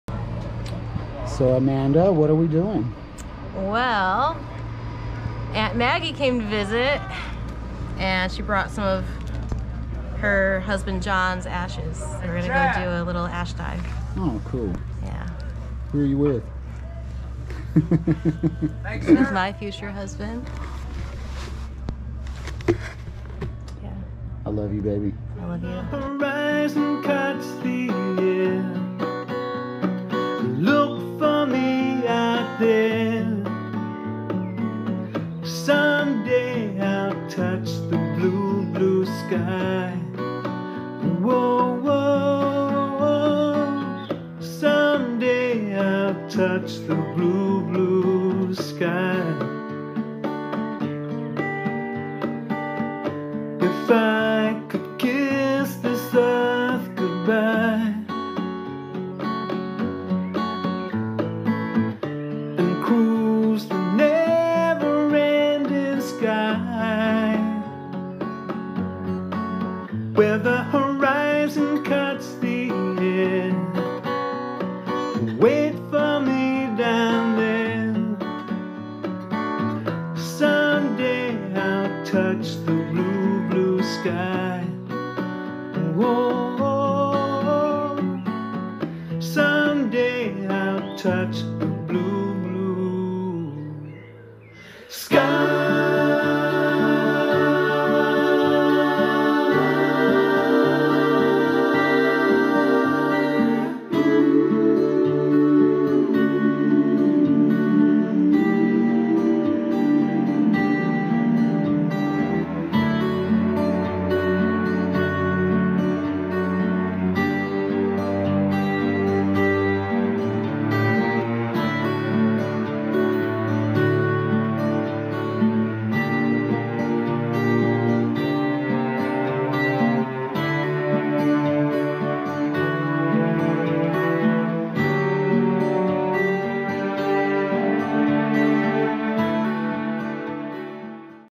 John B. Lewis touches the blue, blue sky.
Aunt Maggie came to visit us at Skydive Arizona during the USPA Nationals and told us an endearing story about how her late husband, John, always wanted to fly. So Amanda and I took some of his earthly remains on a jump from 13K feet over the AZ desert and celebrated his memory the way we skydivers do... with an ash dive. May the Grand Architect rain blessings upon you and all of those who knew and loved you, John... you will never be forgotten.